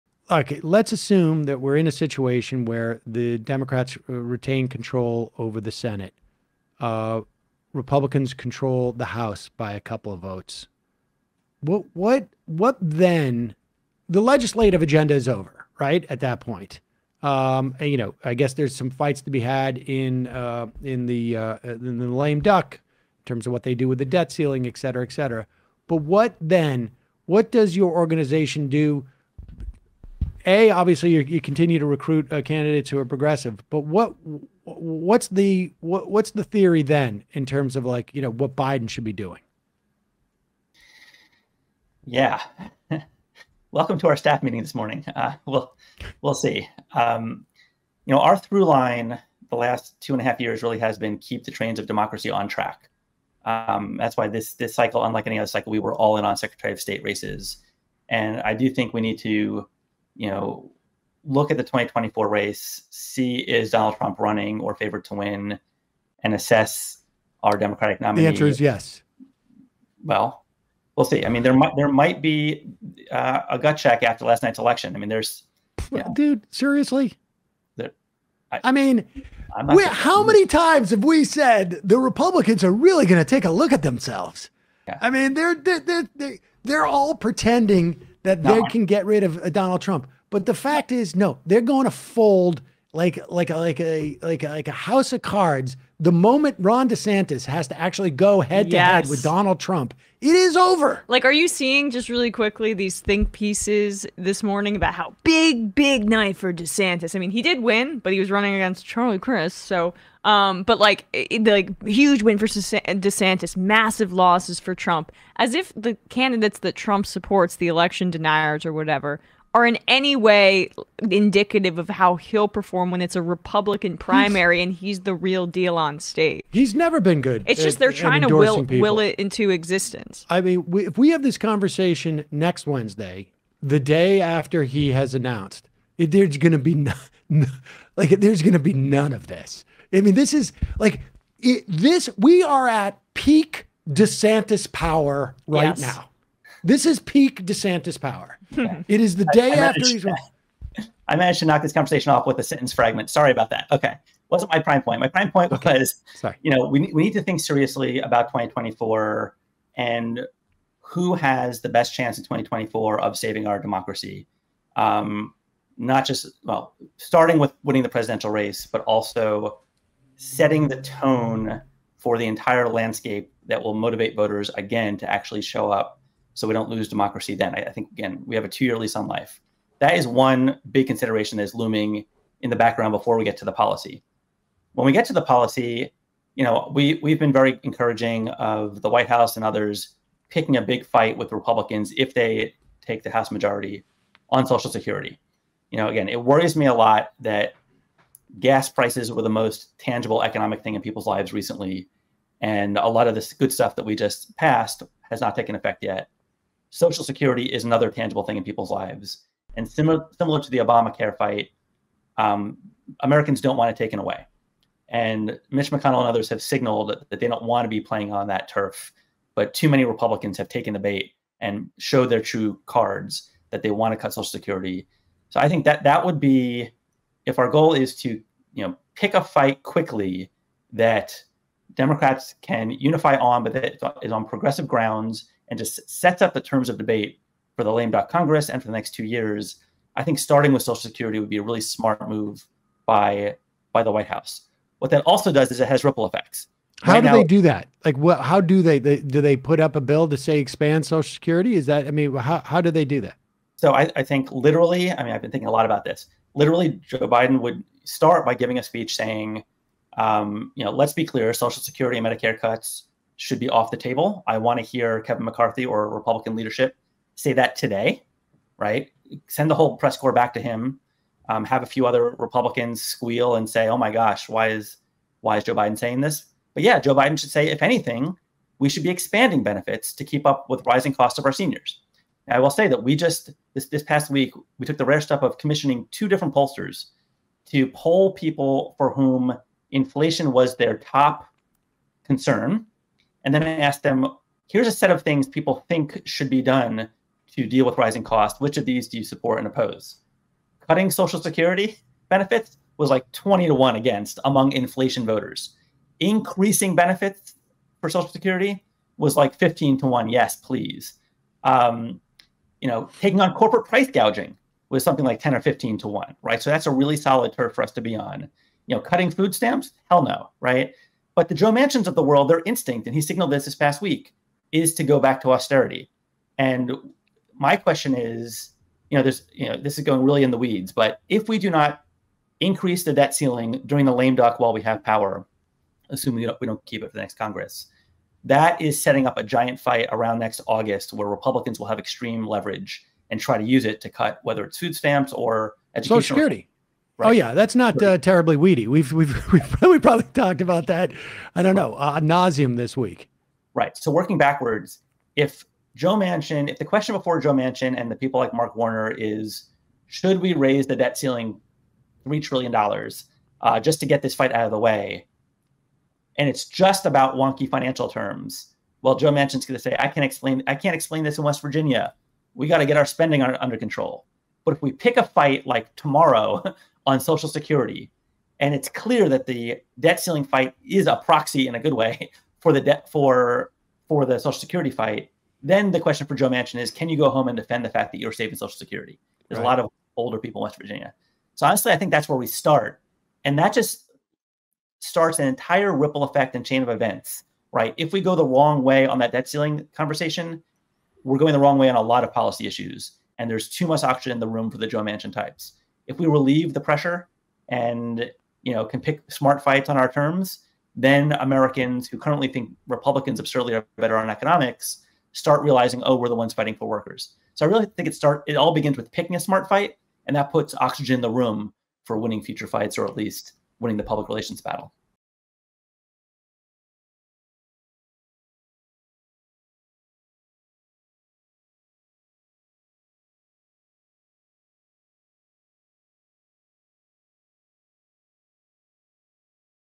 Democrats Already Have Their Winning Issue To Defeat Trump In 2024
If Democrats want to have a winning strategy for the 2024 election all they have to do is run on Social Security. They should run not just on protecting Social Security but increasing benefits. This would put the Republicans back on their heels immediately.

Image has been cropped and color has been altered.

Okay, let's assume that we're in a situation where the Democrats retain control over the Senate. Republicans control the house by a couple of votes. what what what then the legislative agenda is over right at that point. and you know I guess there are some fights to be had in the lame duck in terms of what they do with the debt ceiling et cetera et cetera. but what then what does your organization do obviously you continue to recruit progressive candidates. but what's the theory then in terms of like you know what Biden should be doing. Welcome to our staff meeting this morning. we'll see you know ours through line the last two and a half years really has been keeping the trains of democracy on track. That's why this cycle unlike any other cycle we were all in on Secretary of State races. and I do think we need to you know look at the 2024 race to see is Donald Trump running or the favorite to win. and assess our Democratic economics. well, we'll see I mean there might be a gut check after last night's election. I mean there's yeah. dude seriously? I mean how many times have we said the Republicans are really going to take a look at themselves? I mean they're there there they're all pretending that they can get rid of Donald Trump. but the fact is no they're going to fold like like a like like like a house of cards the moment Ron DeSantis has to actually go head down with Donald Trump. it is over. like are you seeing just really quickly these think pieces this morning about how big knife or DeSantis? I mean he did win but he was running against Charlie Christ. so but is like a huge win versus DeSantis. massive losses for Trump. as if the candidates that Trump supports the election deniers or whatever are in any way indicative of how he'll perform when it's a Republican primary and he's the real deal on stage. He's never been good, it's just they're trying to make it into existence.